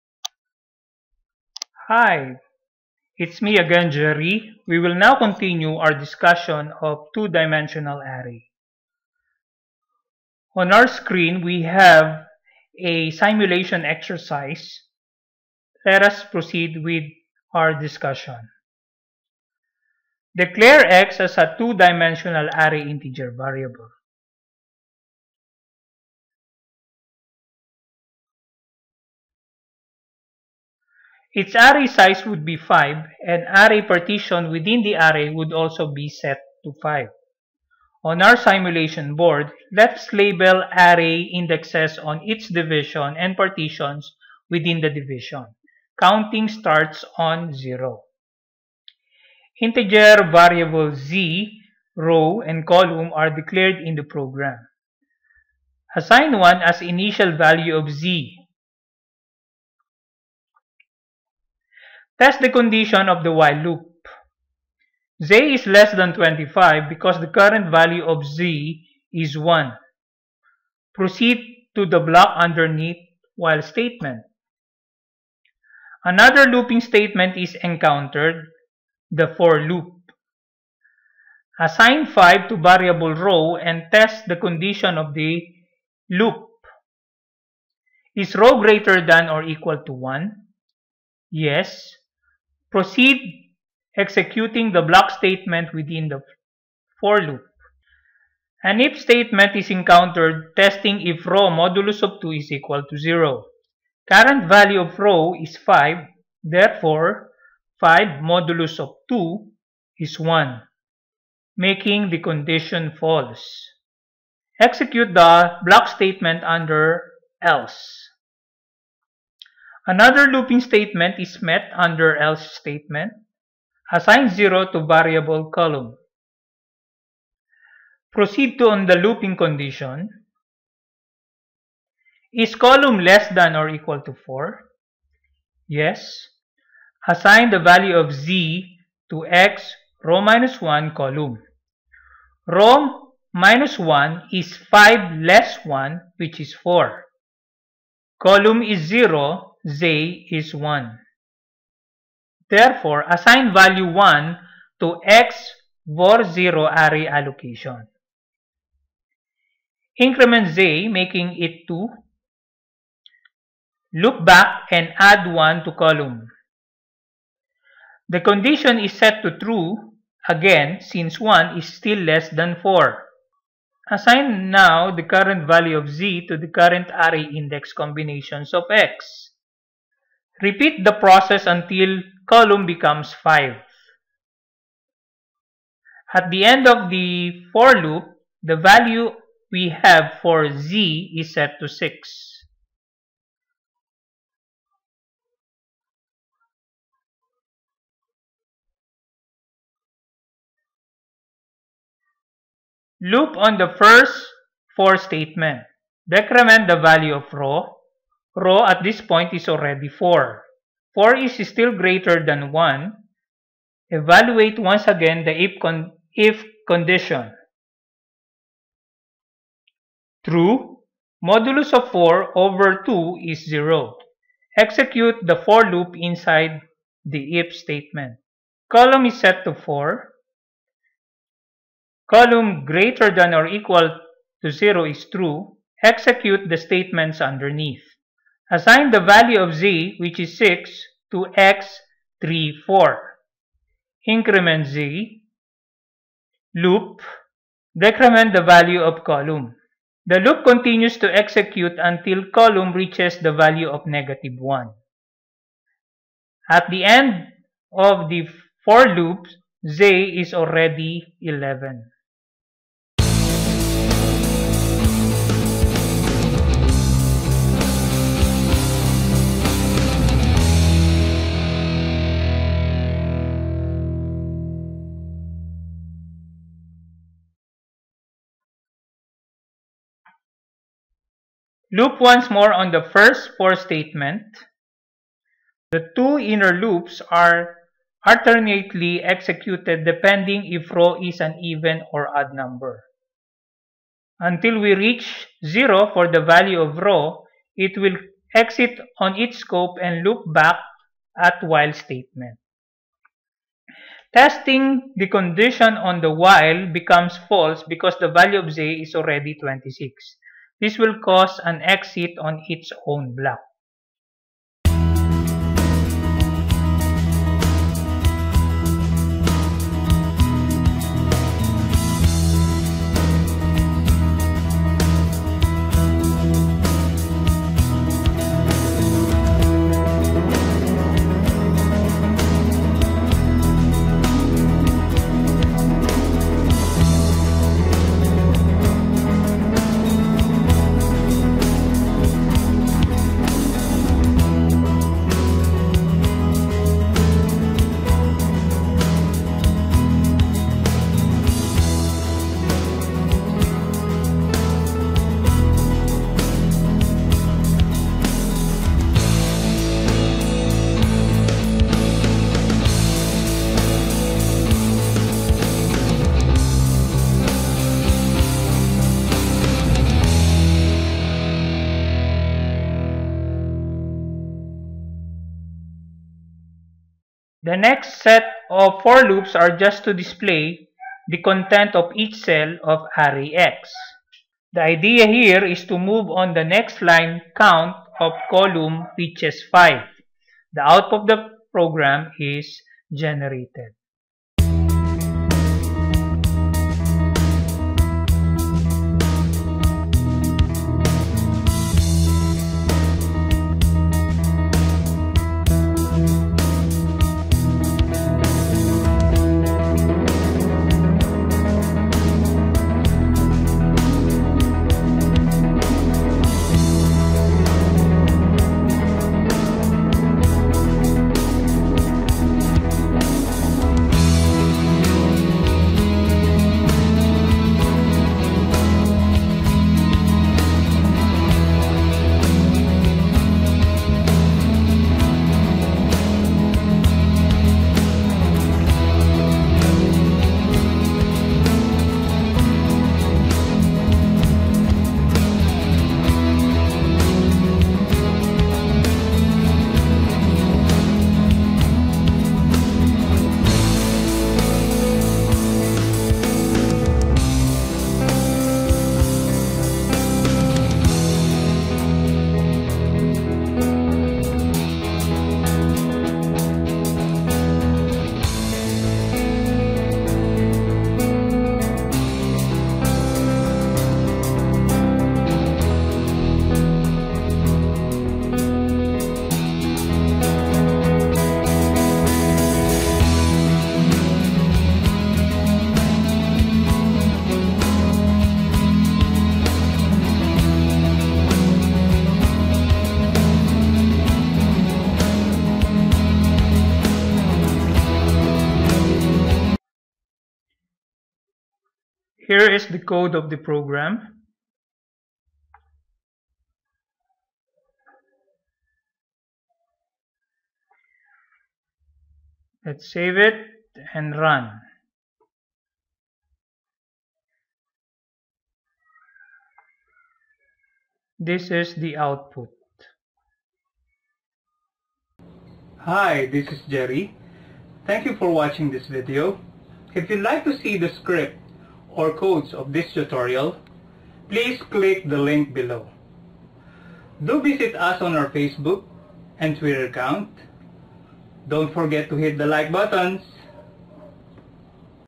My First In Java Tutorial 31: Two Dimensional Array Simulation Example
Detailed explanation of two dimensional array data structure with a given example.